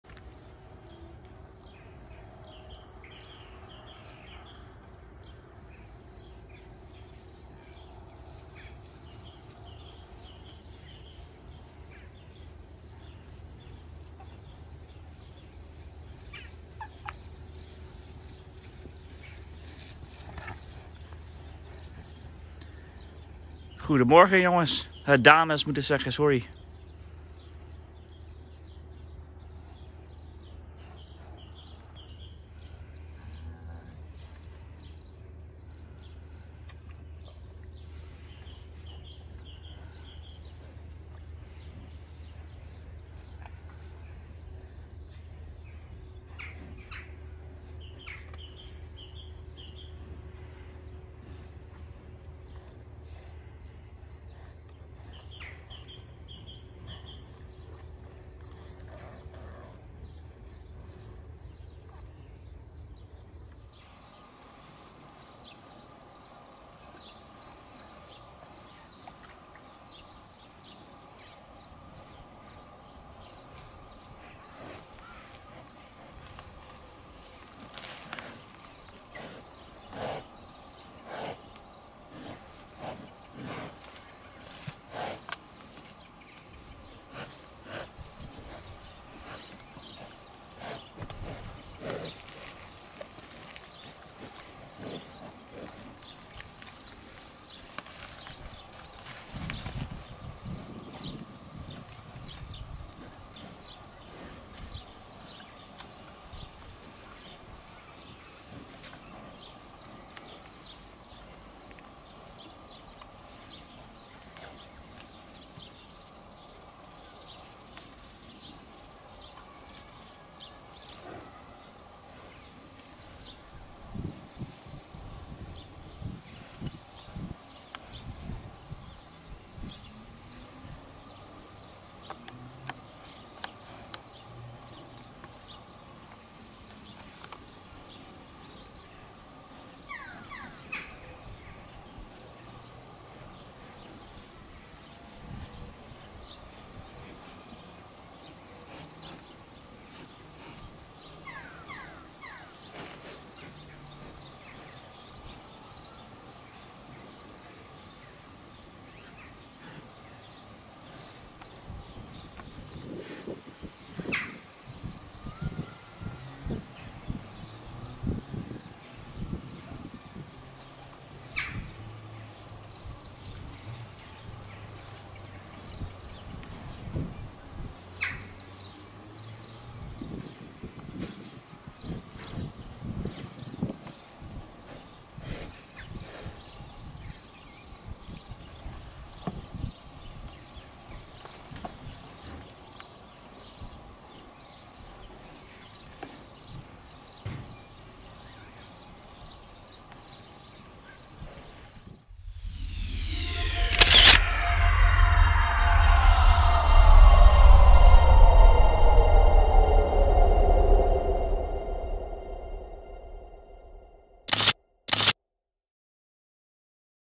Cows Running in Pasture
Milk cows running in pasture. These black and white cows, the classic milk cow of The Netherlands and many parts of the world are called Holstein Friesian cattle. I was walking the dog when I came across a place I often make pictures. The cows started running to me. Amazing how fast these large milk cows are! They are very curious these grass eaters! I thought one of the Friesian cows was going to get pushed in the water, but these are a strong breed of domesticated cattle!

“Holstein Friesians are a breed of dairy cattle originating from the Dutch provinces of North Holland and Friesland, and Schleswig-Holstein in Northern Germany. They are known as the world's highest-production dairy animals.” Source

Wow, another Dutch invention that is used all over the world, if you could call cattle that!

Milk cows running the pasture, Holstein Friesian cattle showing their curious side!